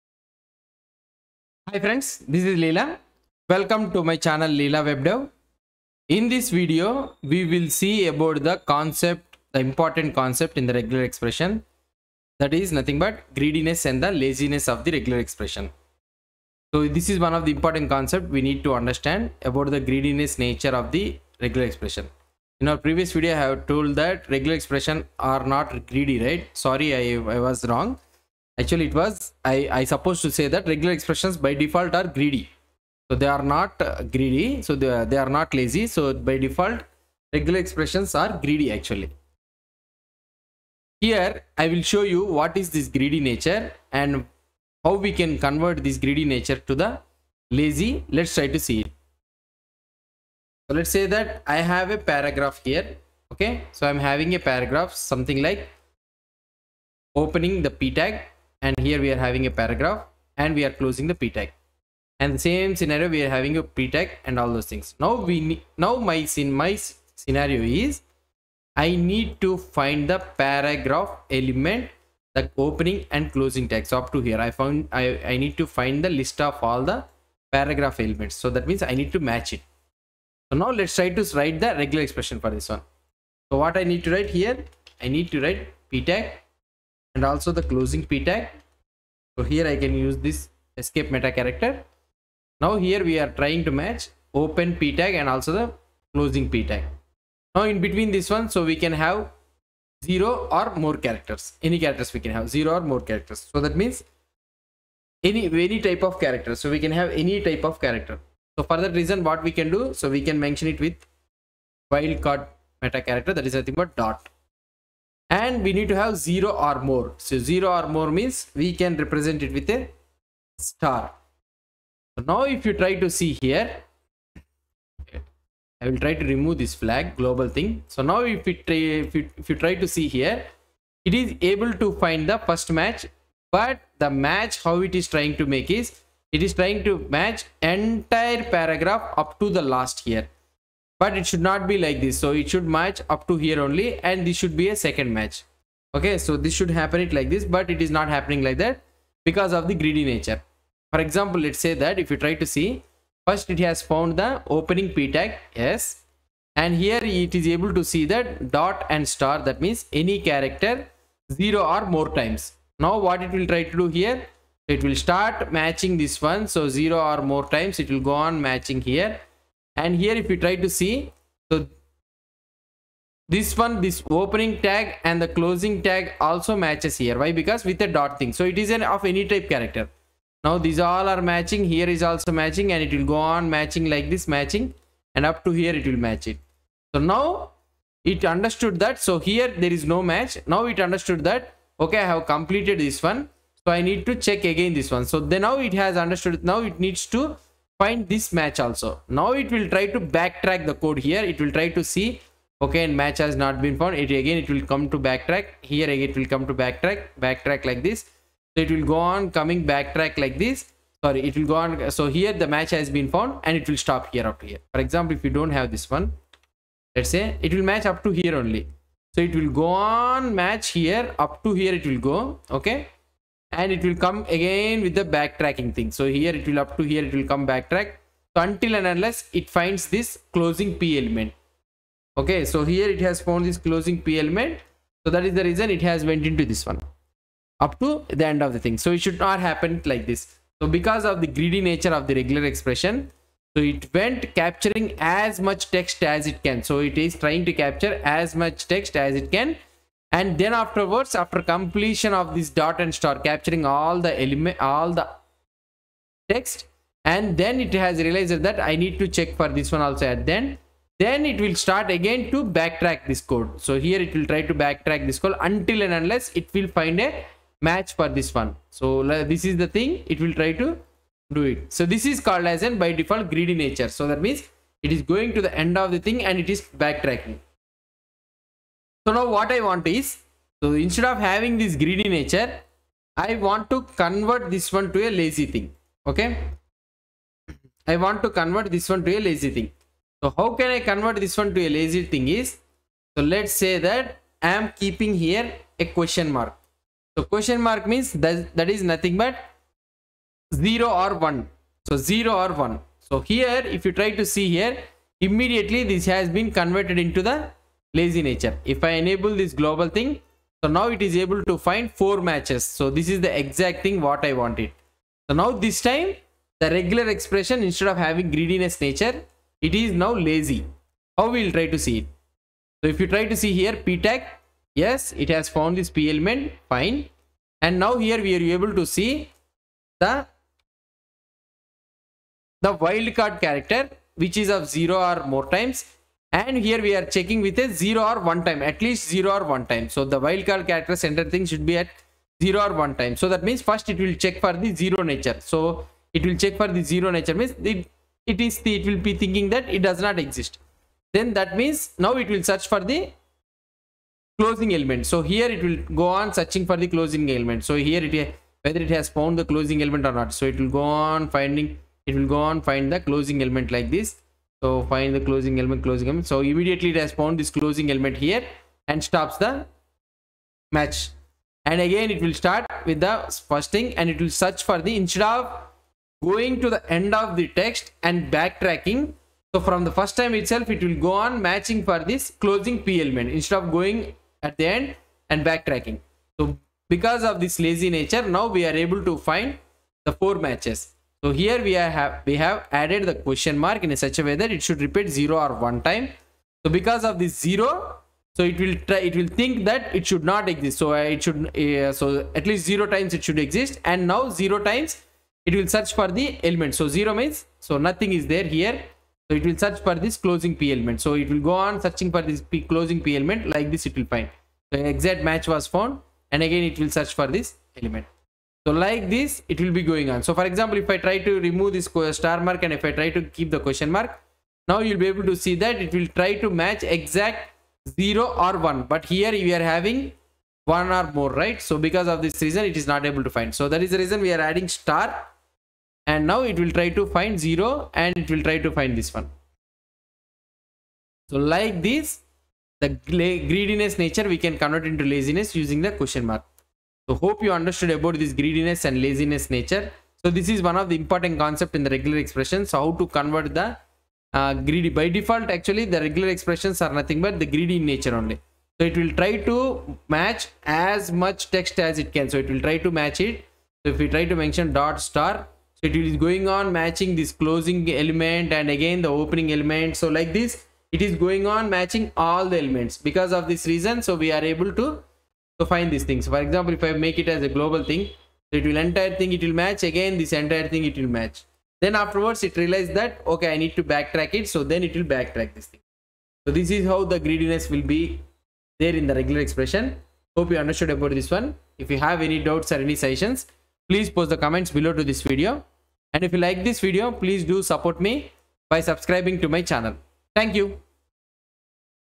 14. Understanding the Greediness and Laziness in the Regular expressions - RegEx
In this video we will see how the greediness and the laziness works in the Regular Expression - Regex.